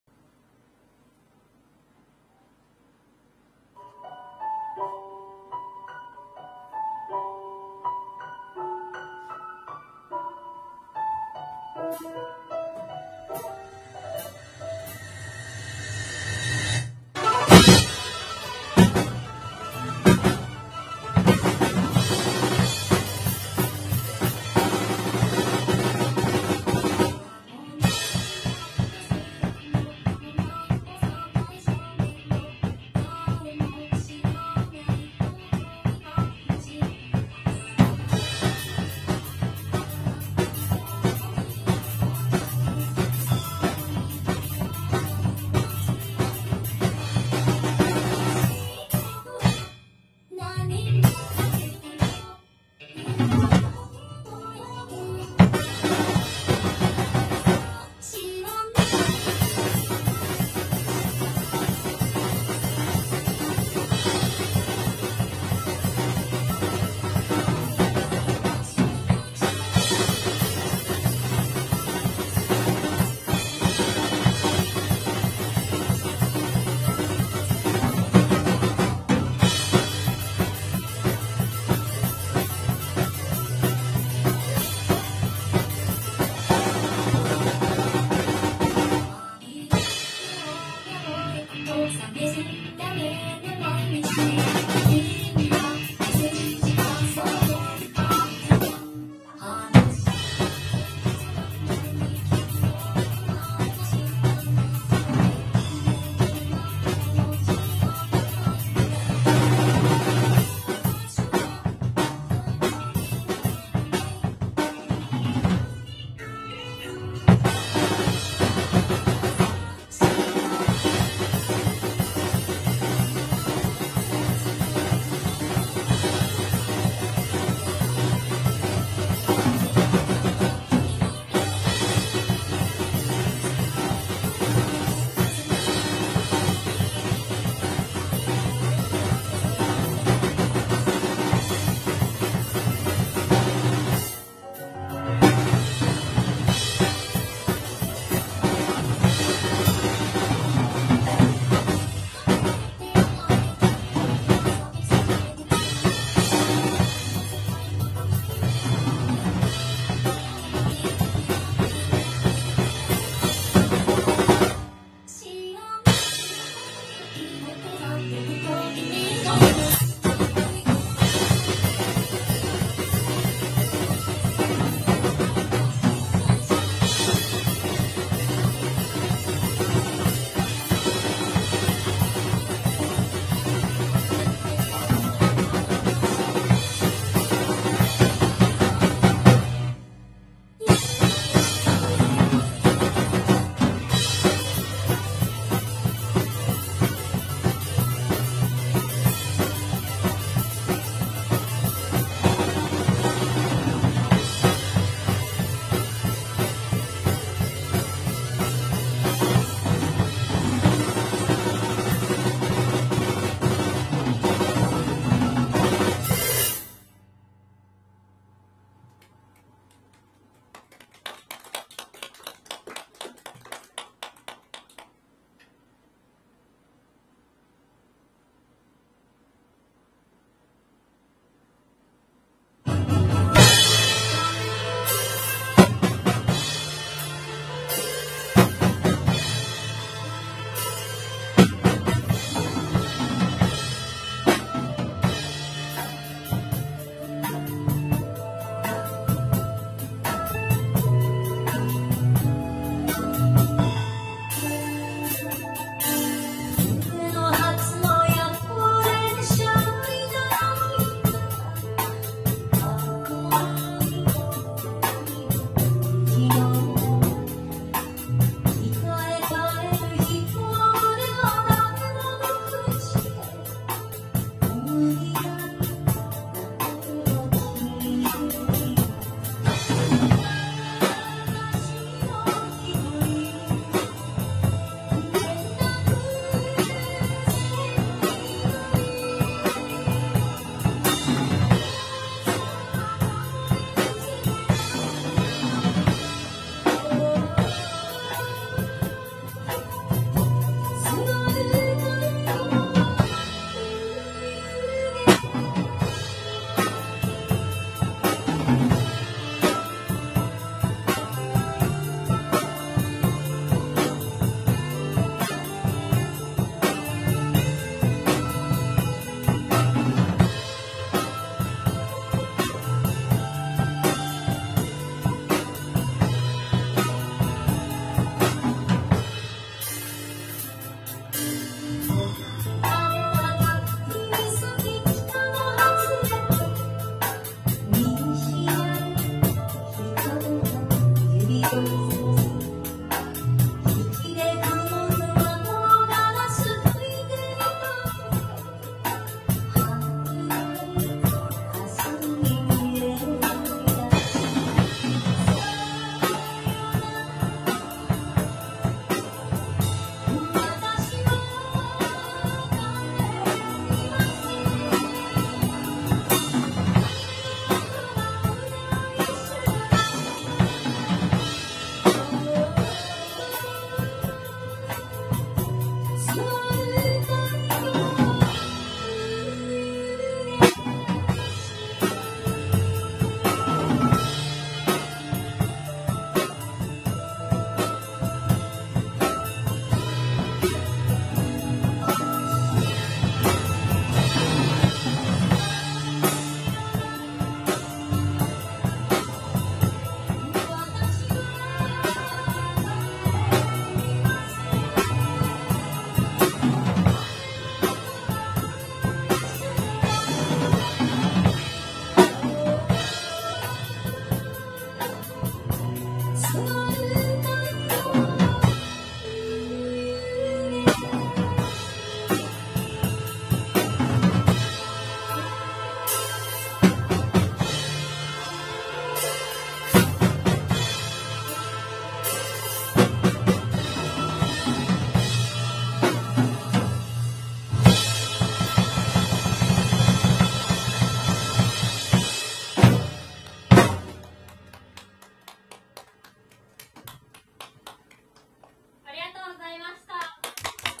「まきぽん」HOTLINE2019 2019.8.11島村楽器水戸マイム店ショップライブ
2019年8月11日に行われたHOTLINE2019水戸マイム店ショップライブです。